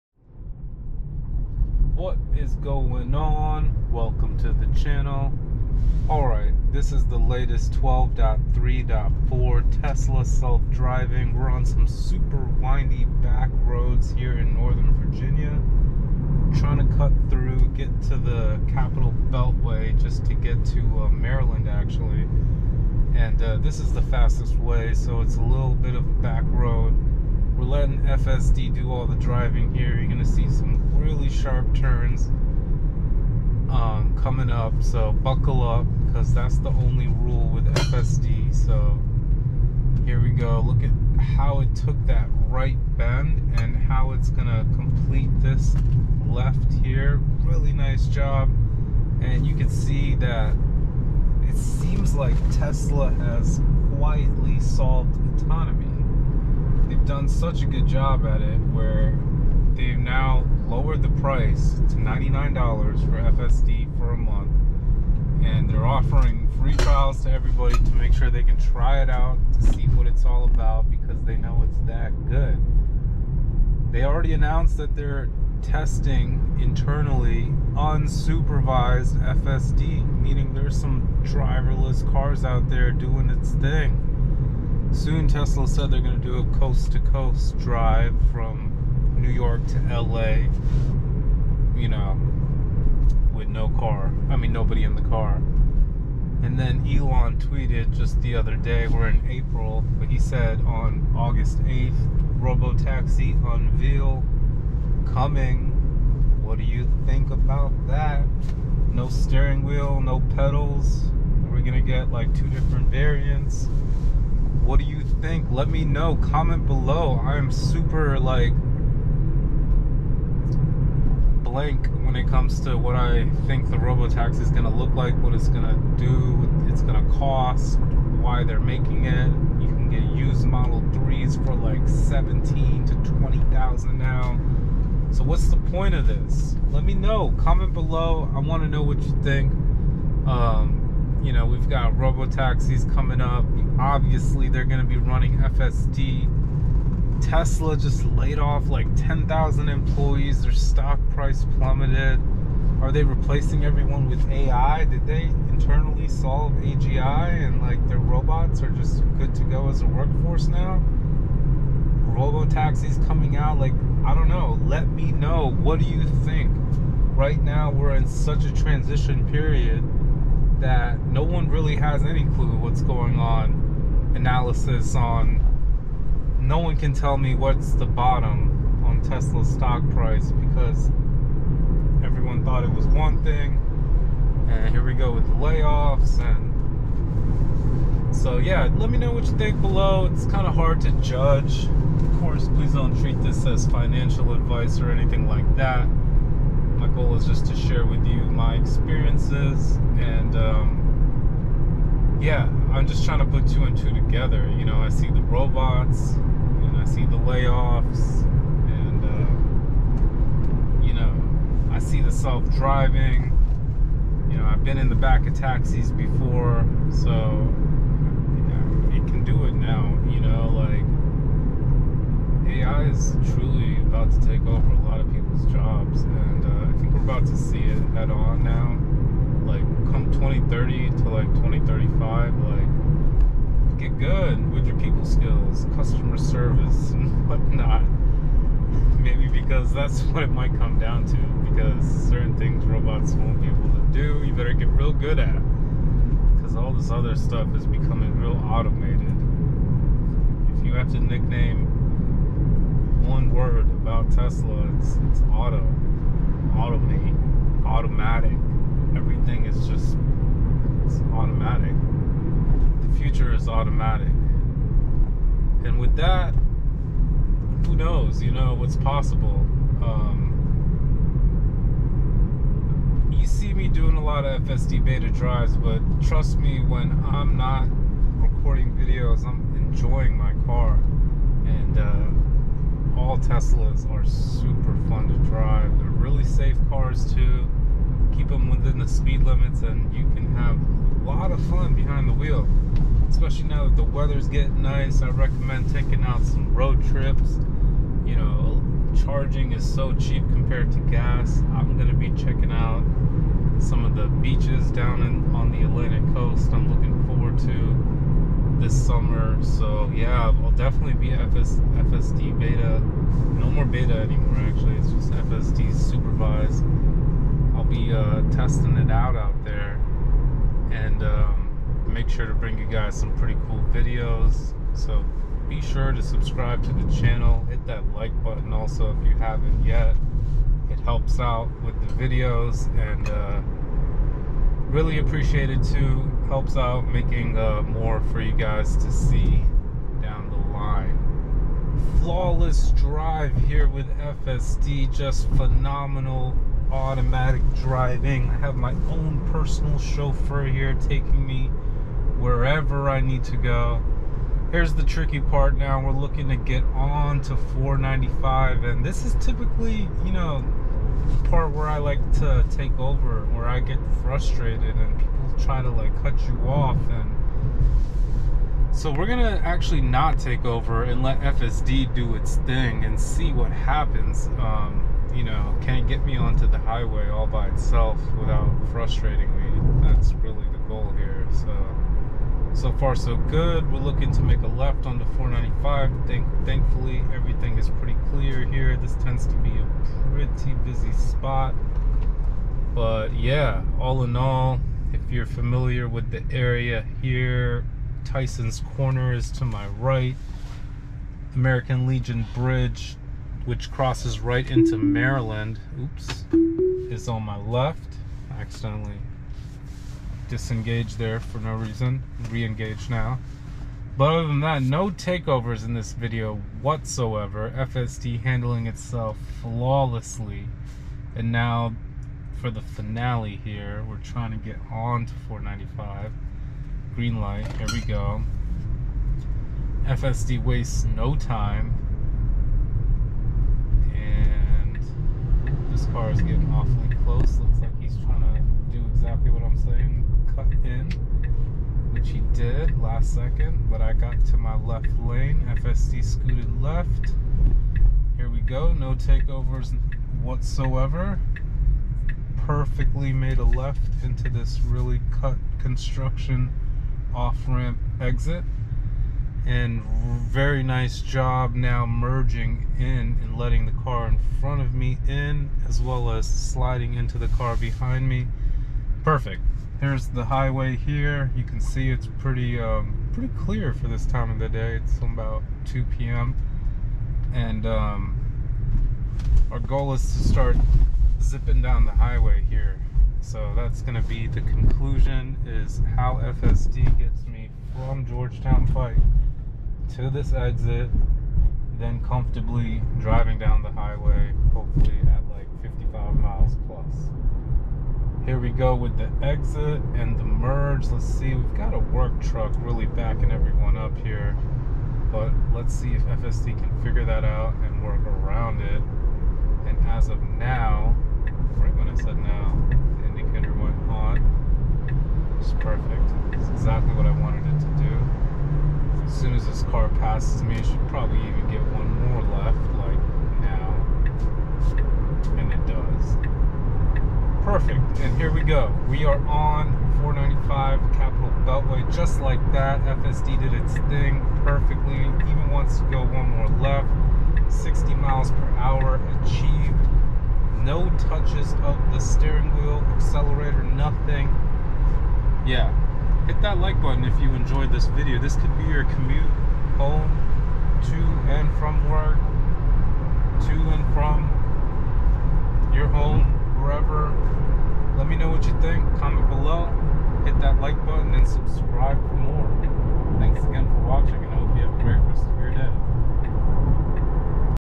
Tesla Robotaxi is coming 10k employees laid off? FSD 12.3.4 priced at $99/mo
Should I buy FSD or Enhanced autopilot?

Auto park is available with Enhanced auto pilot and Full self driving

12.3.4

This is a real life Testimony on Full self Driving

Witness the Chauffer skills of FSD v12.3 as I take it on a spin for the first time to see what its all about and how they Implemented AI into its code

2tb SanDisk Sentry upgrade

Tesla Experience
Enhanced Autopilot
Traffic Visualizations
Computer Vision